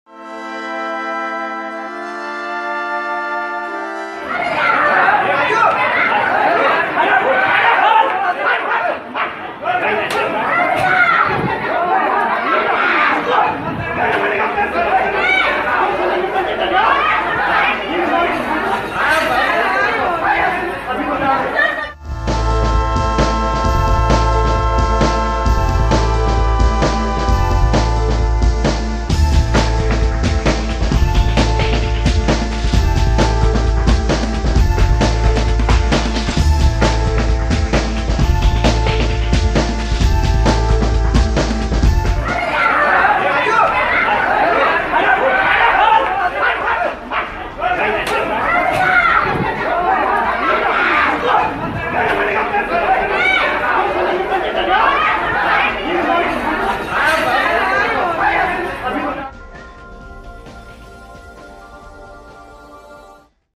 On cam: Junior doctors thrash patient's attendant in Kanpur's LLR Hospital
LLR Hospital in Uttar Pradesh's Kanpur is yet again making headlines for all the wrong reason.